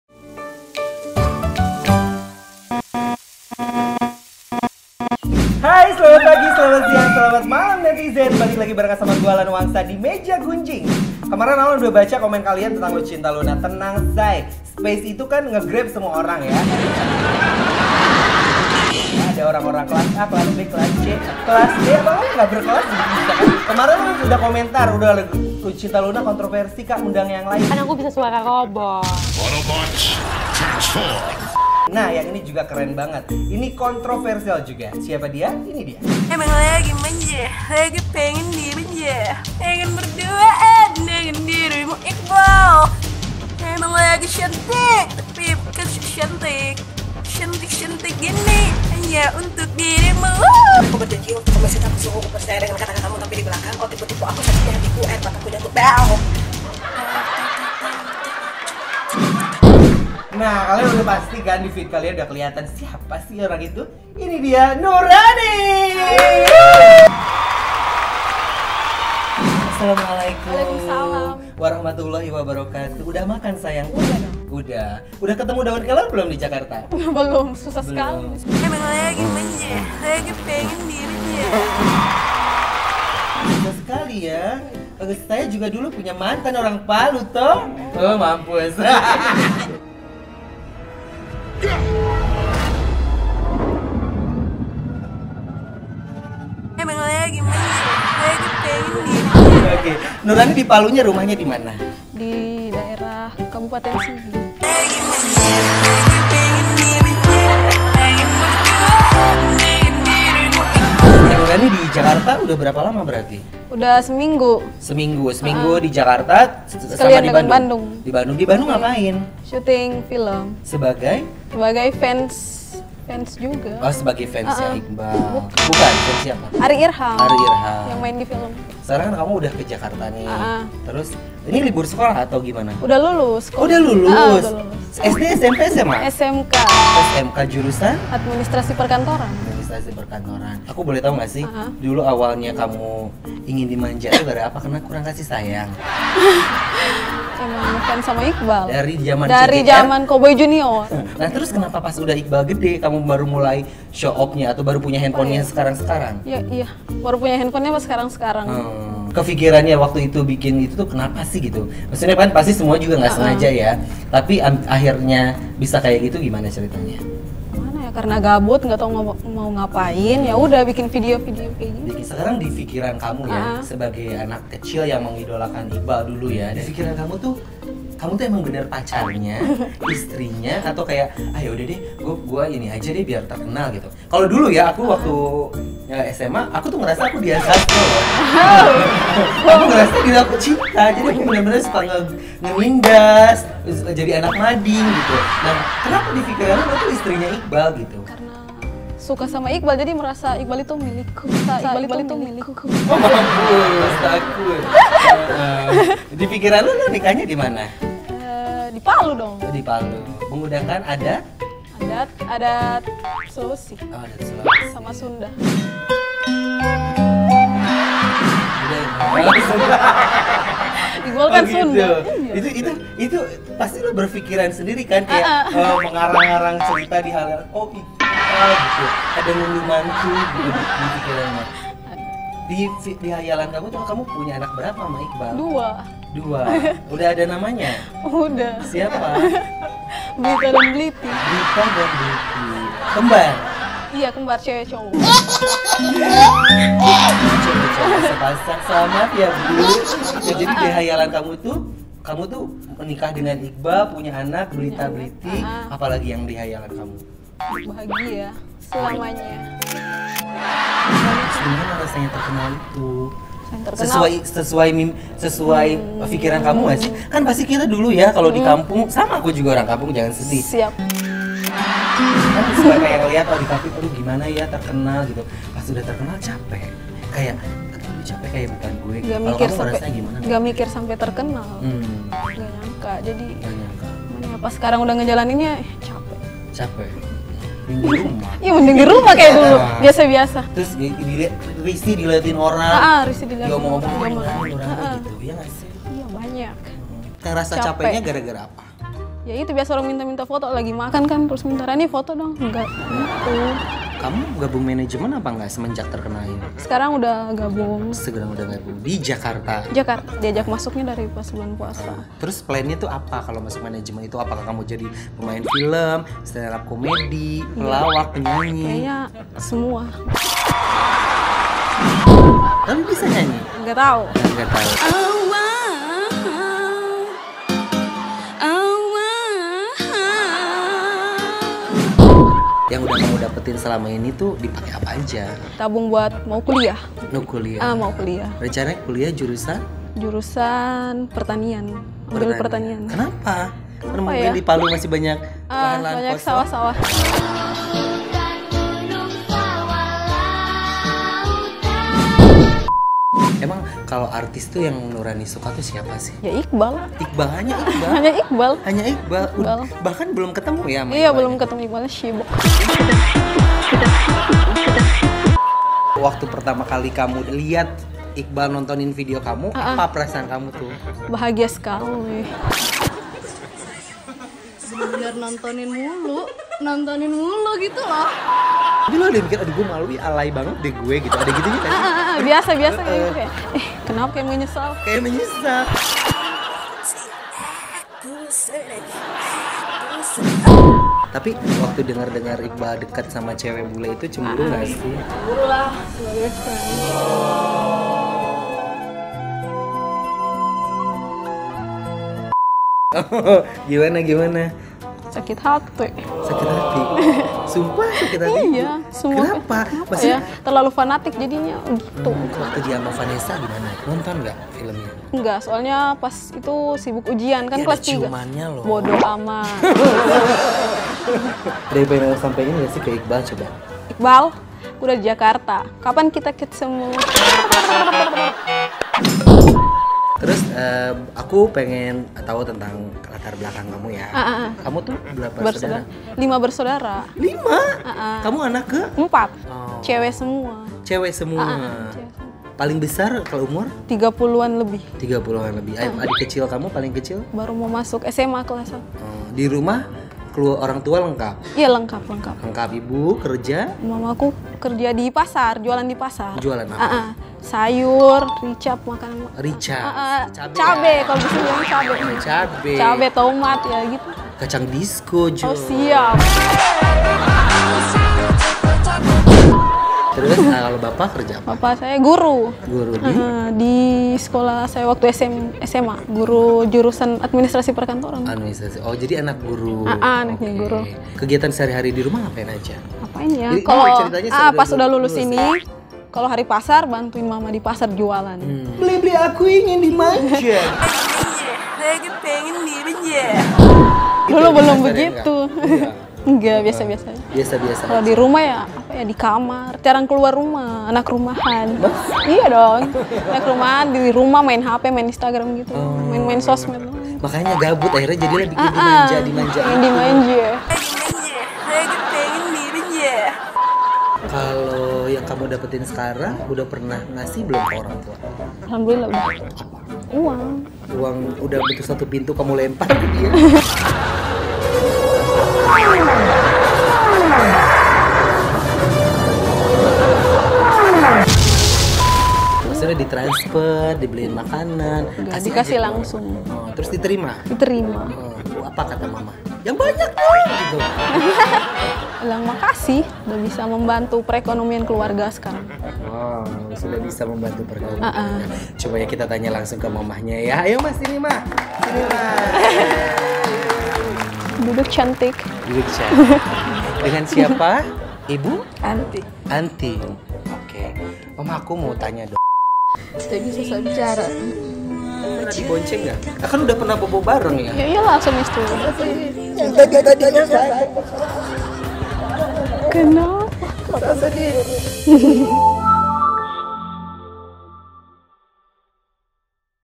[MEJA GUNJING] - MENGUAK SOSOK BLITA & BLITI HASIL HUBUNGAN NURRANI & IQBAAL
Gimana sih kehidupan dari seorang Nurrani yang katanya istri sah Iqbaal? Kamu tinggal duduk manis, dan tonton gimana Allan Wangsa mengorek semuanya :)